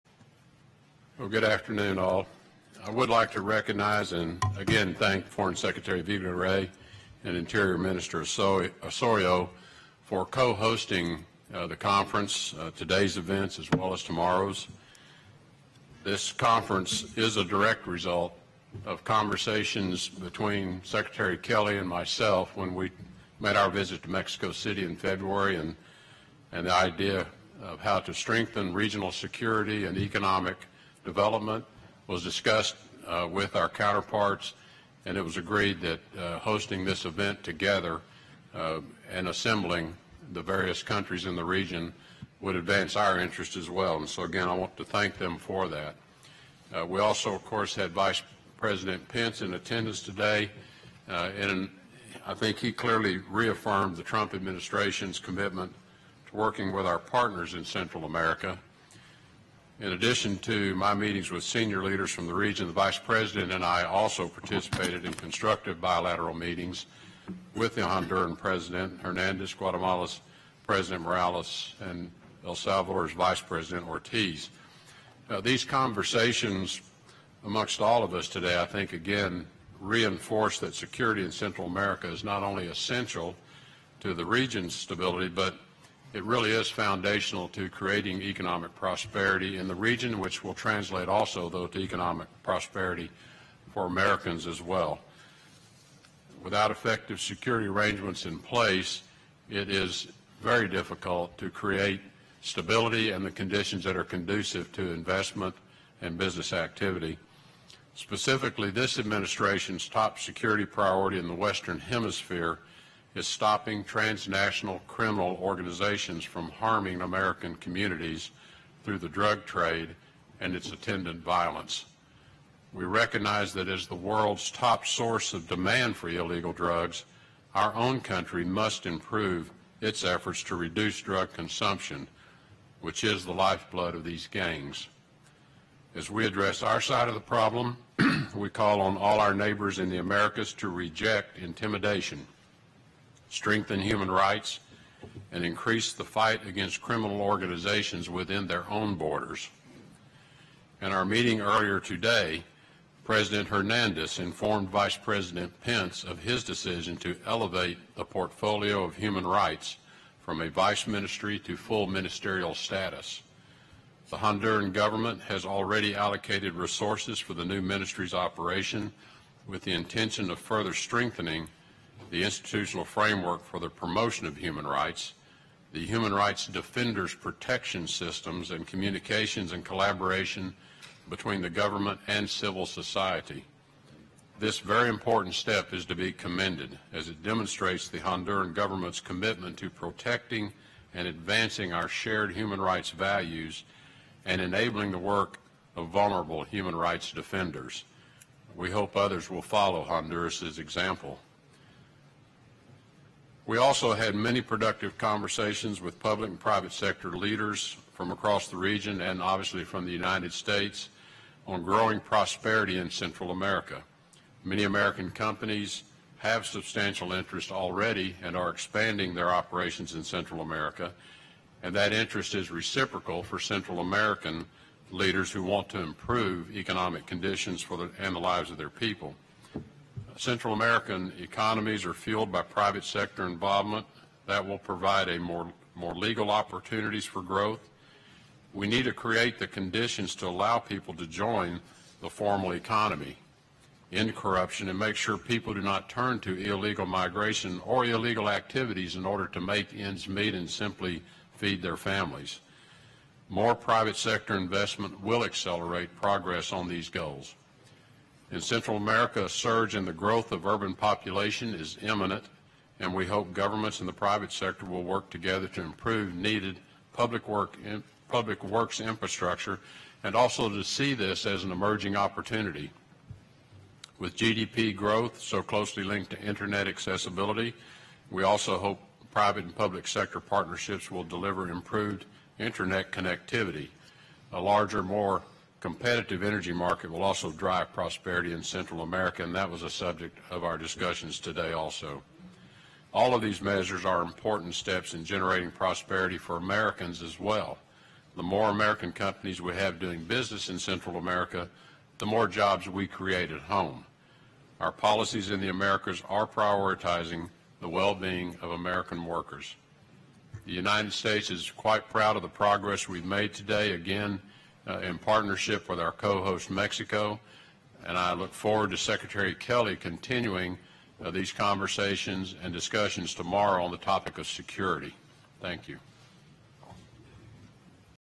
Secretary Tillerson Opens Joint Press Availability with Central American Leaders
Secretary Tillerson participates in a joint press availability at the Conference on Prosperity and Security in Central America, at Florida International University, in Miami, FL on June 15, 2017. He is joined by Mexican Foreign Secretary Luis Videgaray, Honduran President Juan Orlando Hernández, Guatemalan President Jimmy Morales, and Salvadoran Vice President Óscar Ortiz.